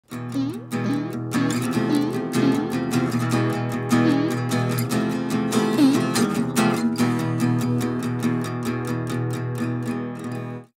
Denny's Dancing Guitar
This video was originally filmed without sound.  Some friends and I recently added sounds to it for a project on Foley editors.  The sounds we created are nearly identical to the original... probably.